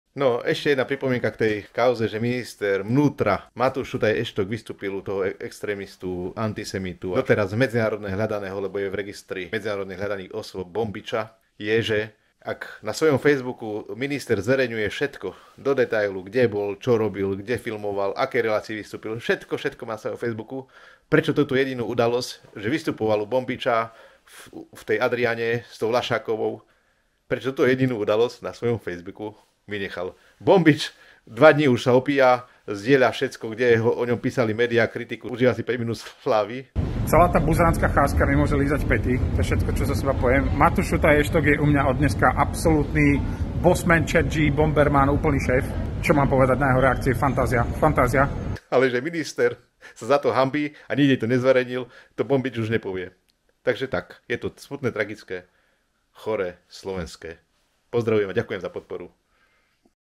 Minister Eštók priznal chybu a k Bombicovi sa nepriznal. 5 minút slávy extrémistu v ľufte
Minister Matúš Šutaj Eštok nezverejnil žiadnu správu o jeho vystúpení v relácii Judity Laššákovej s Danielom Bobicom aka Danny Kollárom a tým priznal, že spravil chybu.
Problém nieje trestne stíhaný Bombič, ale konanie ministra vnútra Eštóka, ked legitimizuje človeka, ktorý sa vyhýba úkonom trstného konania. Vám ďakujem za podporu

Miesto: Bratislava
Osoby: Martin Daňo
Iné: megalodon, gorila, nakrúcanie, politika, vláda, parlament, SMER-SD, demokracia, totalita, smrť, genocída, národ,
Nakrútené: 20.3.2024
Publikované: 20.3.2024
Séria: Gorila, Megalodon, Slovensko, Slovakia